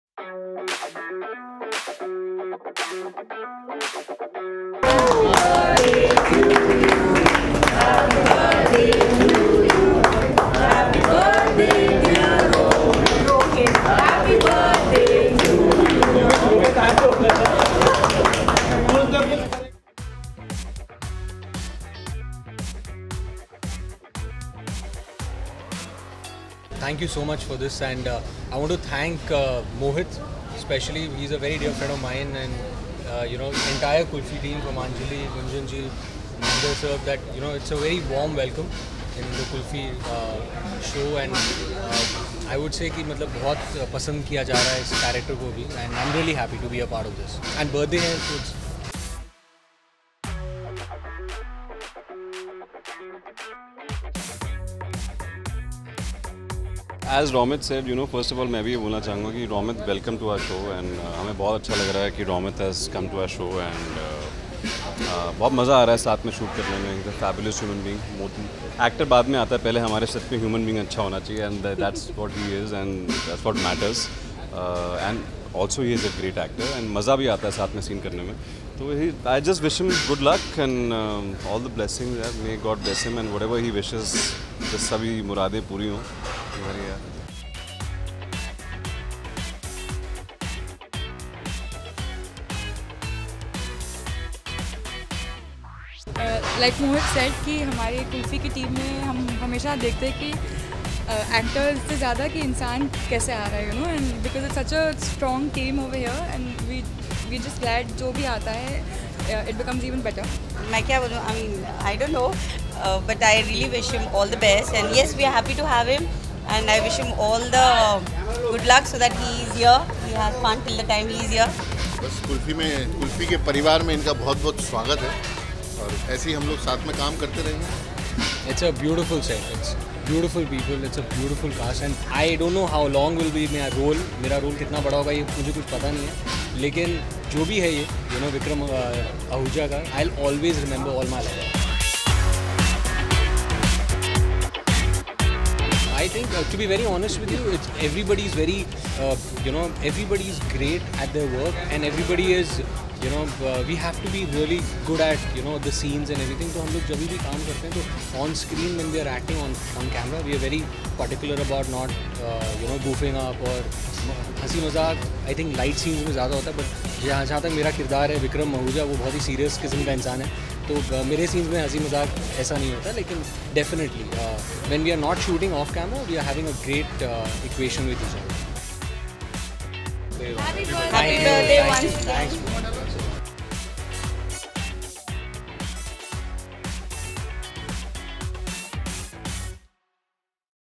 TV actor Romit Raj celebrates his birthday on the sets of Kullfi Kumar Bajewala
Kullfi Kumar Bjewala actor Romit Raj celebrates his birthday this year on the sets  of the show along with actor Mohit Malik and Aakriti Sharma.  Check out The Video To Know More!!